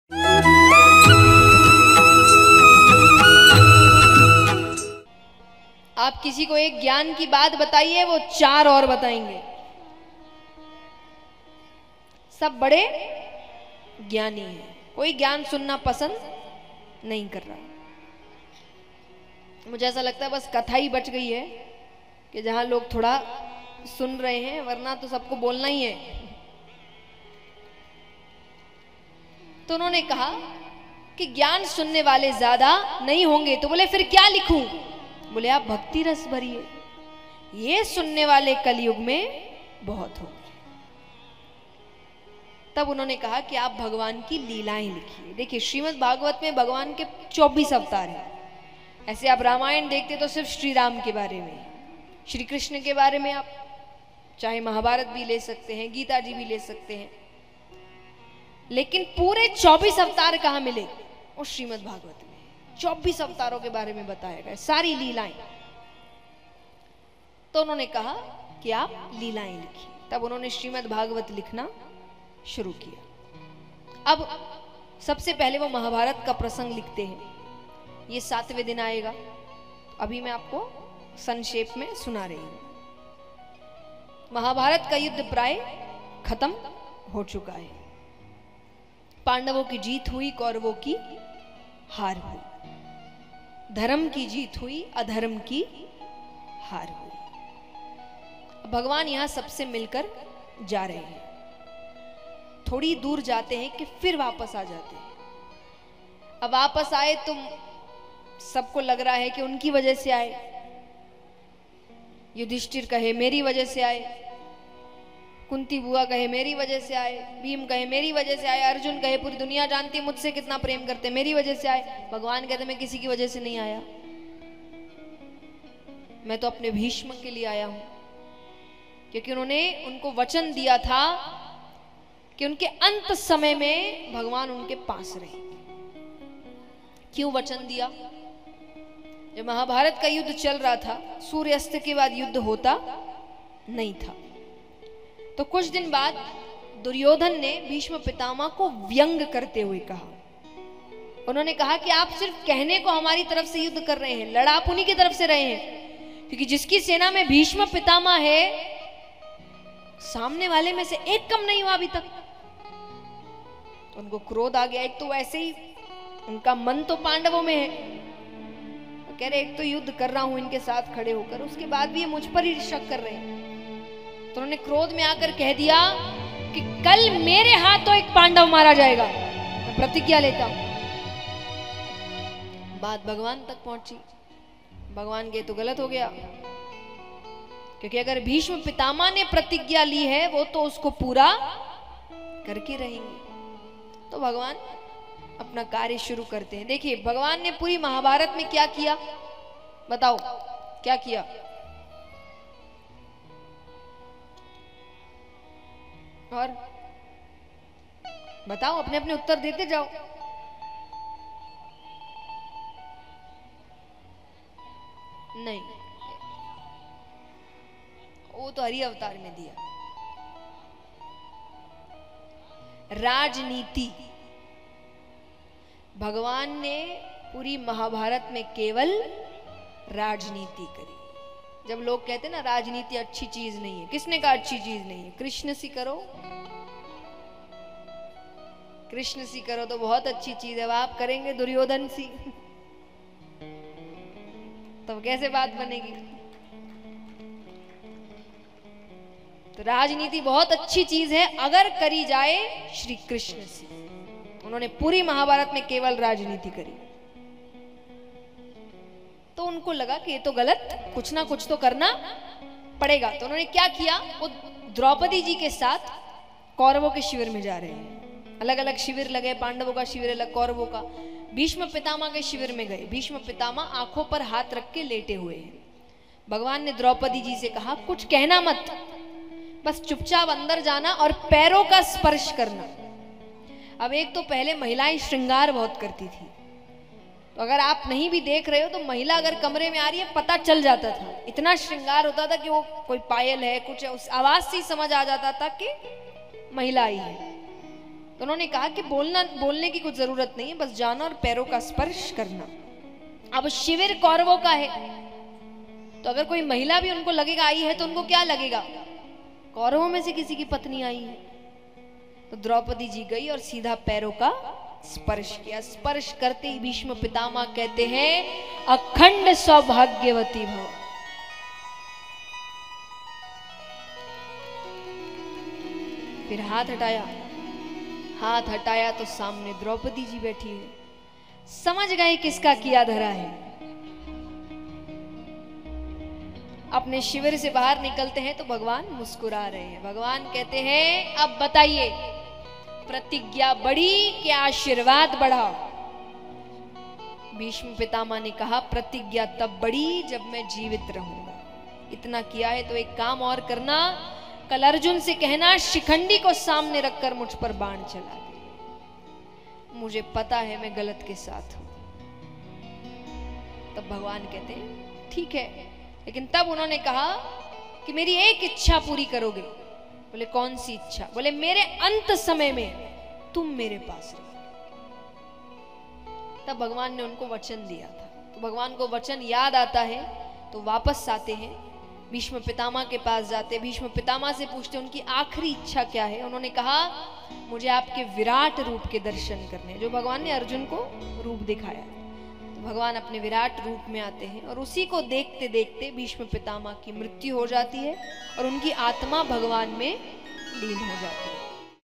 भगवान ने कैसे किया भीष्म पितामह के साथ छल ? Jaya Kishori Ke Pravachan | Mahabharat Katha
भगवान ने कैसे किया भीष्म पितामह के साथ छल ? Jaya Kishori Ke Pravachan | Mahabharat Katha | Satsang TV